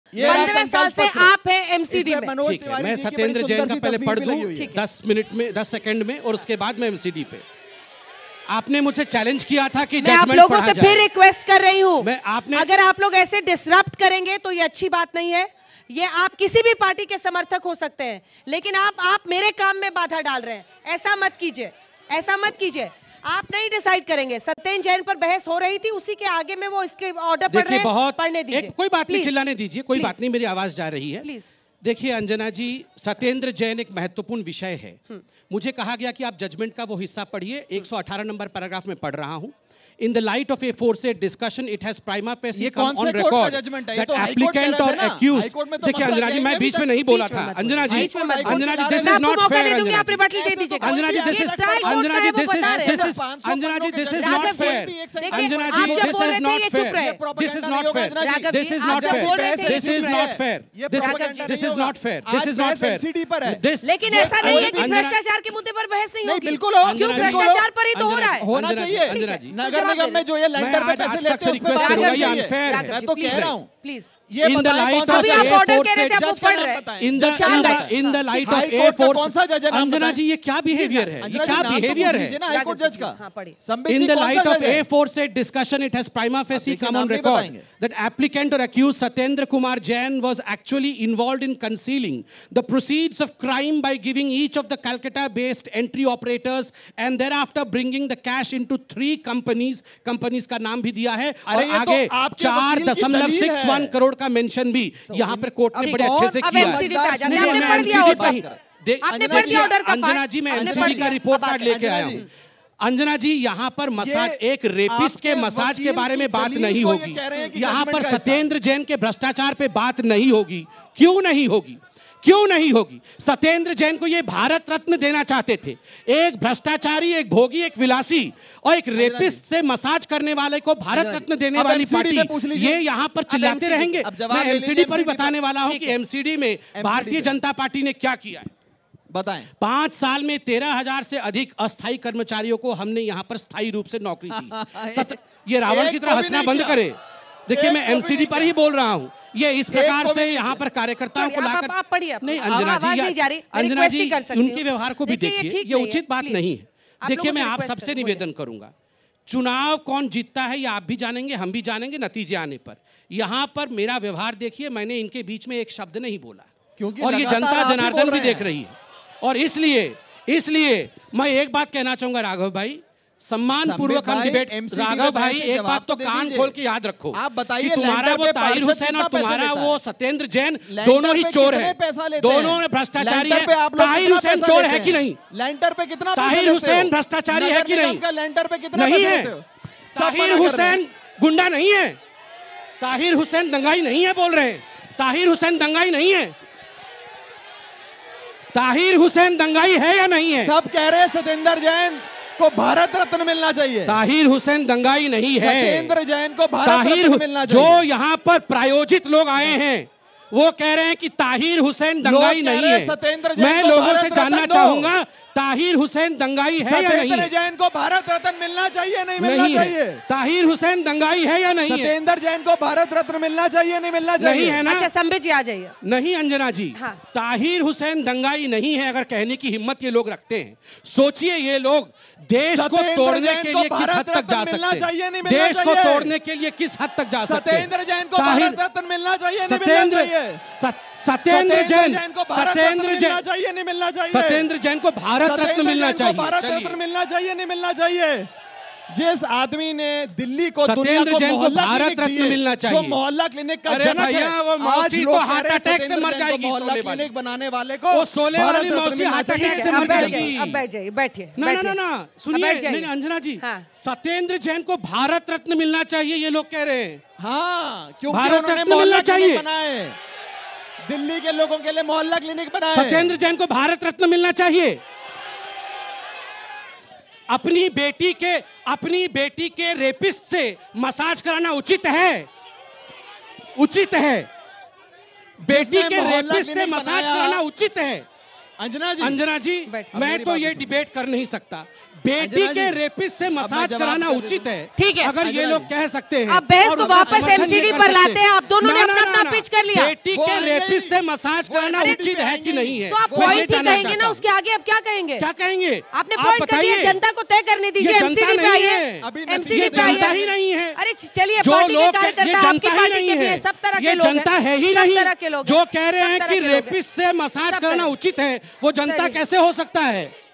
Delhi Panchayat AajTak: Sambit Patra ने कहा- एक रेपिस्ट से आखिर कैसे मसाज करा सकते हैं?
दिल्ली में नगर निगम चुनाव के लिए 250 सीटों पर 4 दिसंबर को वोटिंग होनी है. इस चुनाव के लिए माहौल पूरी तरह से बन चुका है. जीत के लिए सभी दल भी एड़ी चोटी का जोर लगाए हुए हैं. इस बीच आजतक सभी दलों के खास नेताओं को एक मंच पर लेकर आ रहा है. इसी सिलसिले में आज दिल्ली पंचायत आजतक का आयोजन हो रहा है.

Voting is to be held on December 4 on 250 seats for the Municipal Corporation elections in Delhi. The atmosphere has been completely created for this election. All the parties are also trying their best to win. Meanwhile, Aaj Tak is bringing prominent leaders of all parties on one platform. In this connection, Delhi Panchayat Aaj Tak is being organized today.